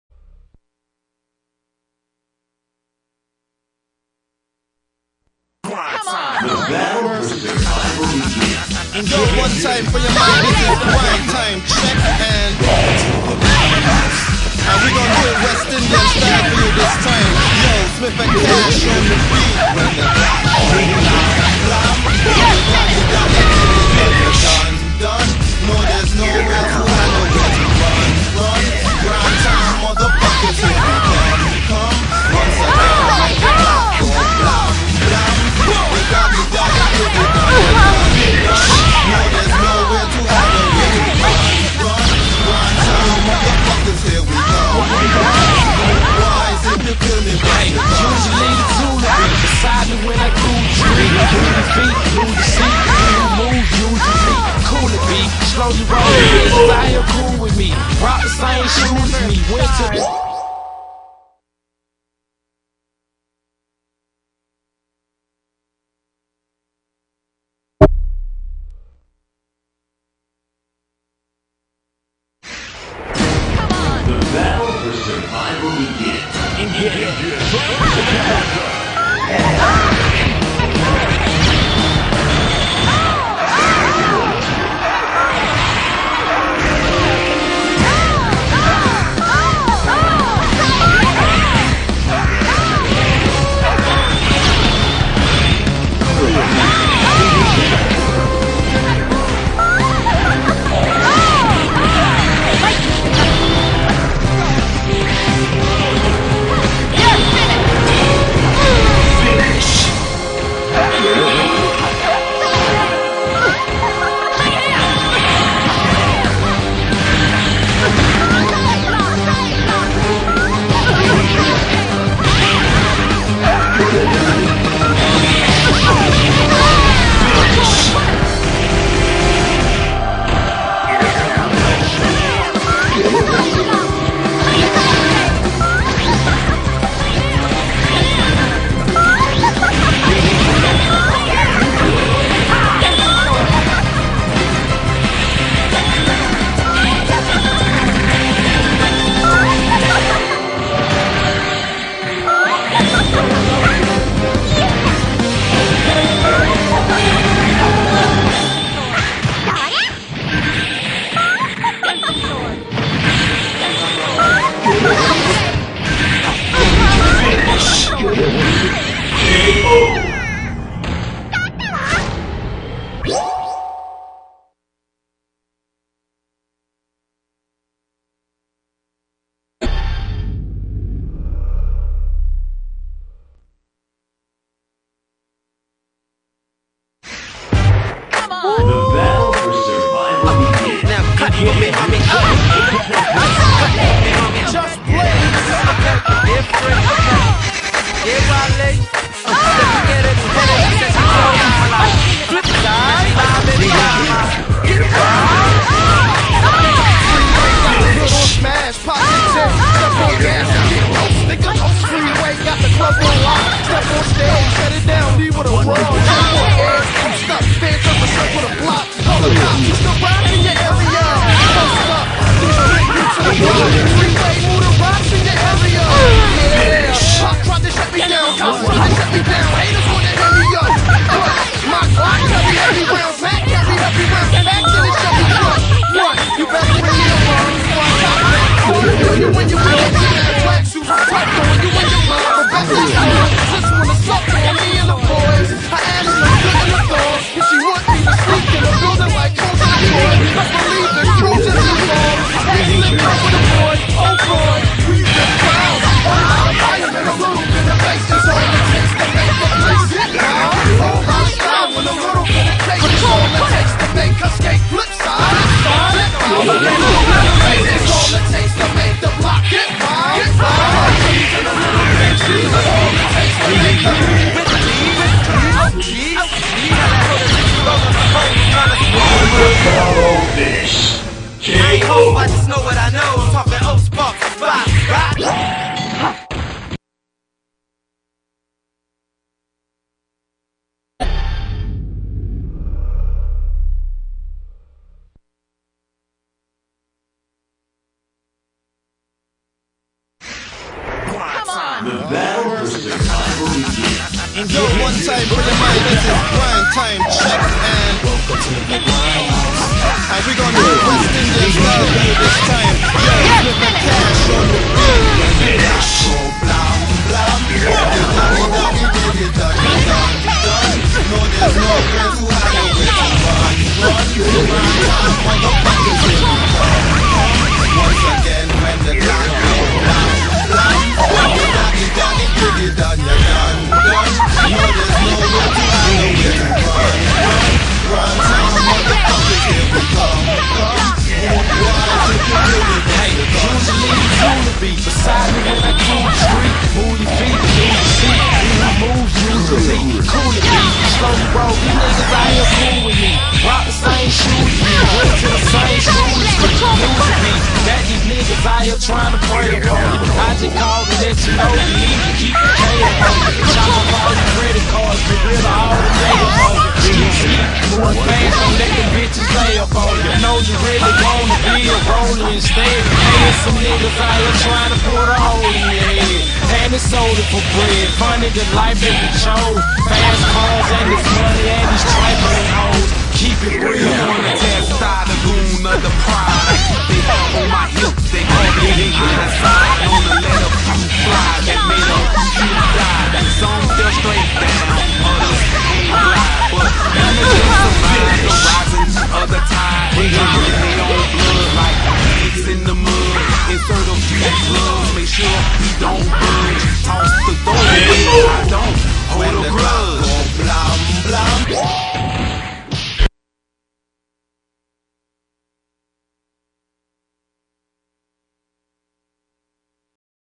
Vidness vs. Pablo all female characters -1
Pabs & I battle using only the female characters...He says "Can I use Storm", I say "Only if I can use Spiral"